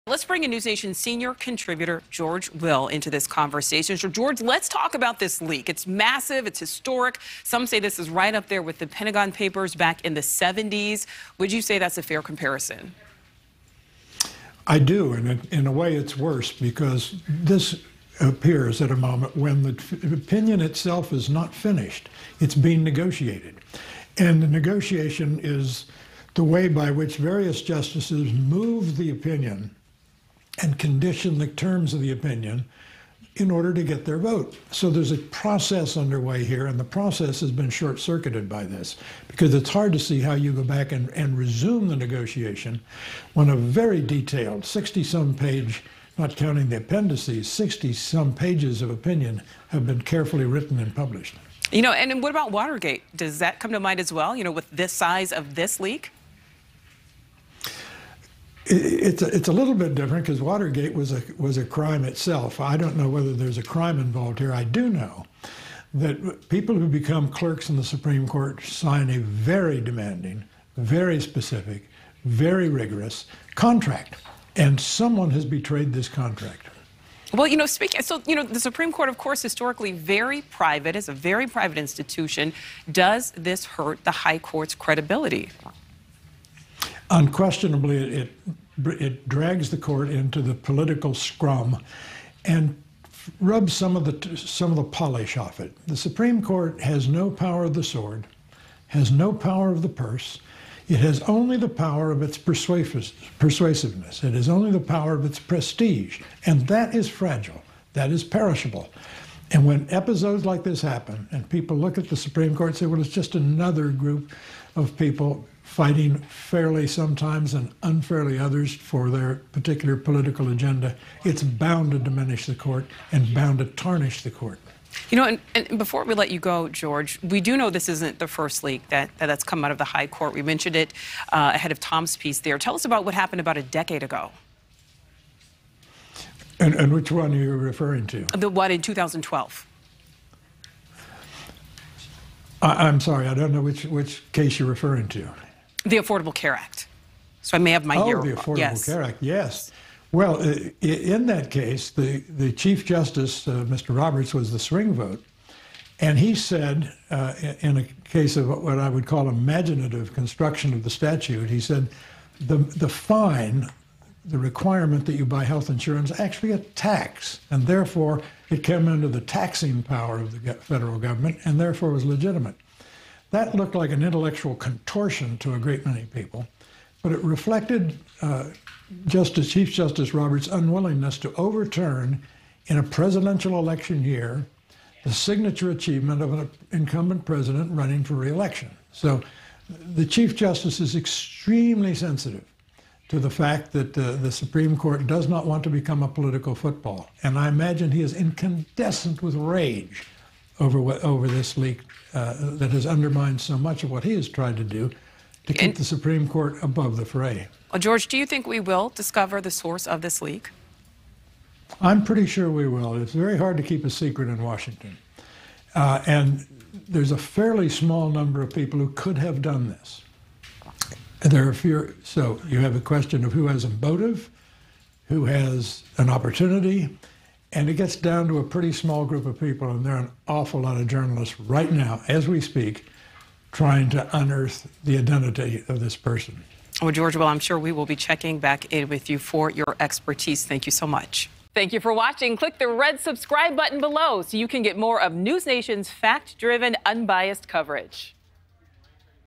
'Someone betrayed that contract' historic impact of Supreme Court leak explored | Rush Hour
It's not quite Watergate or the Pentagon Papers, but Monday's leak of a Supreme Court draft decision will have a major impact on the reputation of the court.